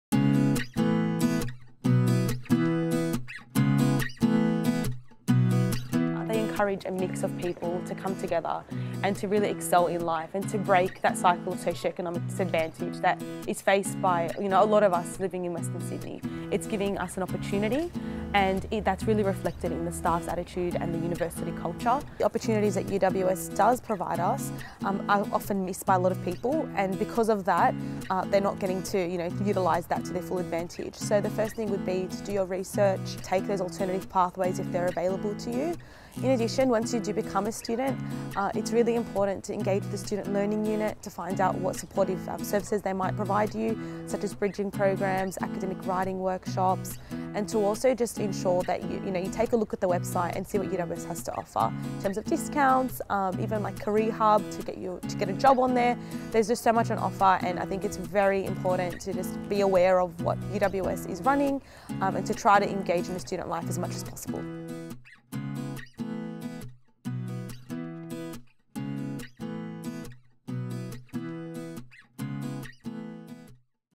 What makes UWS different ?
4th Year Social Sciences Student Rhonda, highlights the countless opportunities and support UWS provides for students.